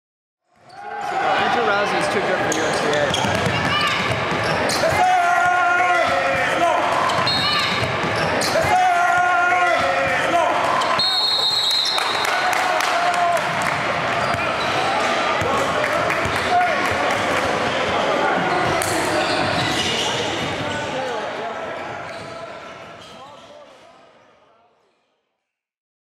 Jalen Tate finishes two-hand JAM @ Nike MDC - All Ohio Red c/o 2016
Jalen Tate finished the break in style with a two-hand jam at the 2014 Nike Memorial Day Classic.